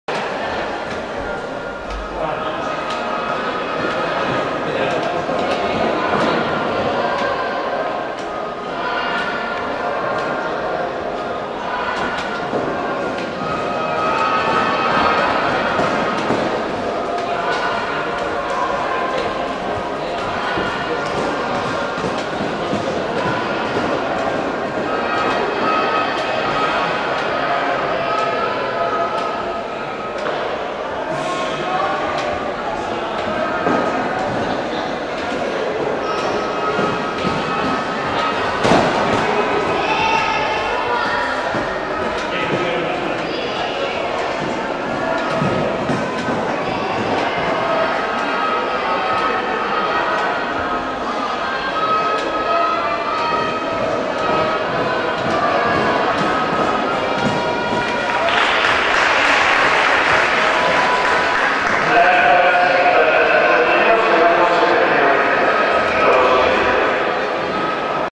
Ainhoa Garcia & Blanca 11.03.17
Concurso Hípico de Salto de Obstaculos Liga Territorial Centro Hípico Los Porches.